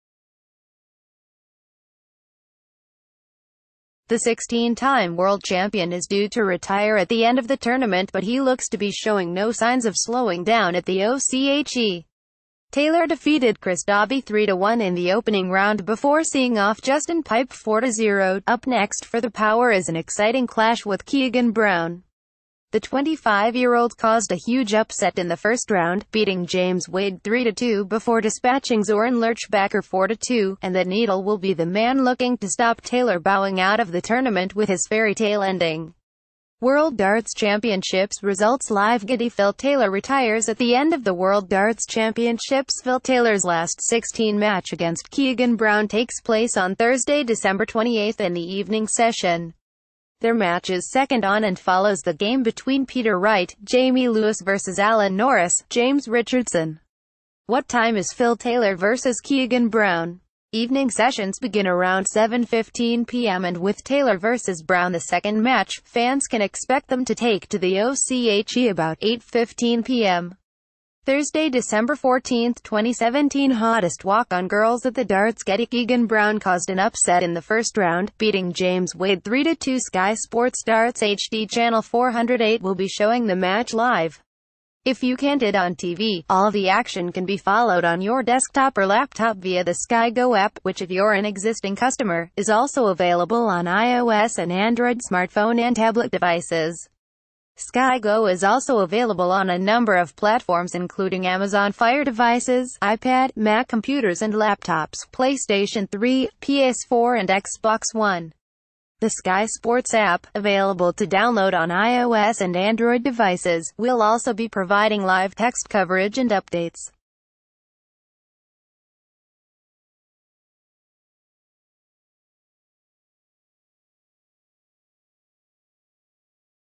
World Darts Championships: When is Phil Taylor playing Keegan Brown in last 16?| Morning News
The 16-time world champion is due to retire at the end of the tournament but he looks to be showing no signs of slowing down at the oche. Taylor defeated Chris Dobey 3-1 in the opening round before seeing off Justin Pipe 4-0.Up next for ‘The Power’ is an exciting clash with Keegan Brown. The 25-year...